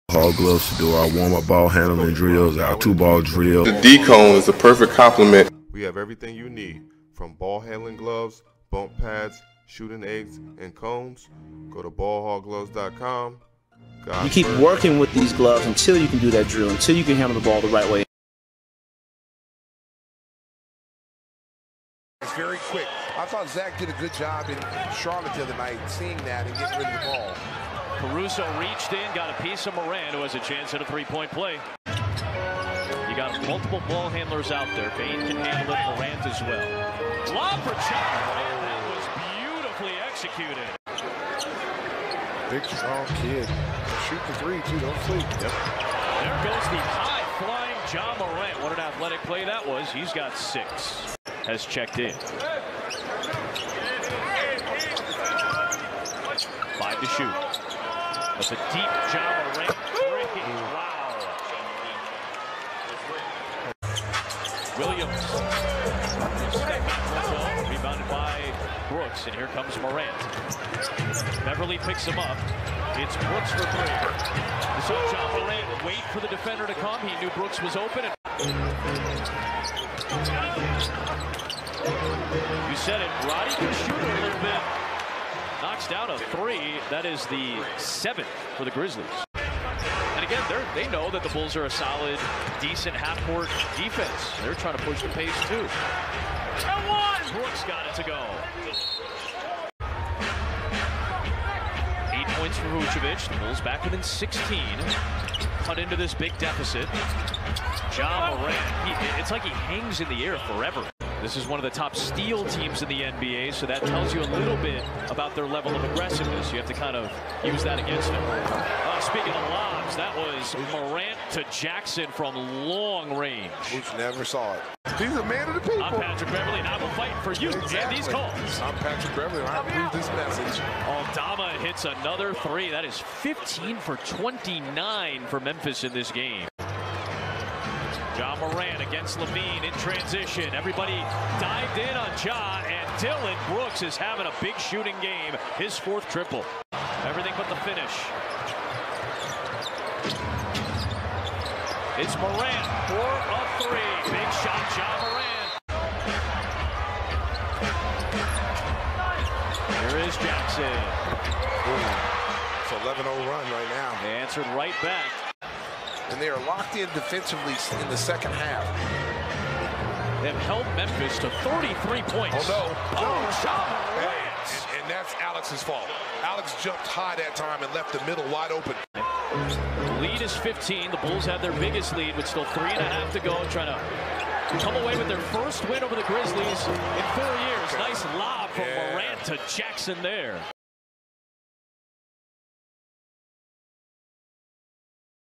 Ja Morant 17 pts 10 rebs 10 asts vs Bulls 22/23 season
For BALL HOG GLOVES Amazon Store